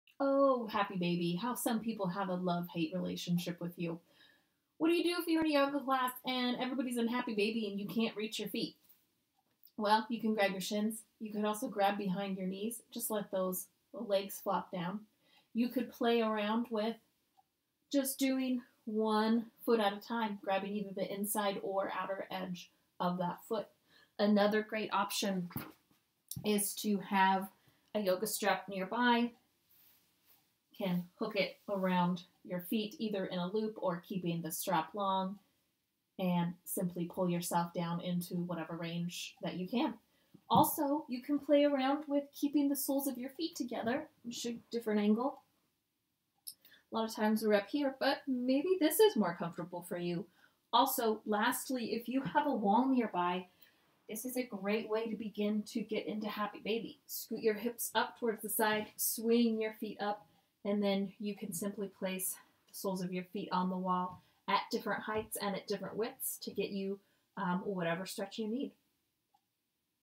Happy Baby Pose Modifications
Can't reach your feet in Happy Baby or looking for alternatives? Try this!

This pose is great to help open the chest, stretch the lower legs, lengthen the spine and release low back pain.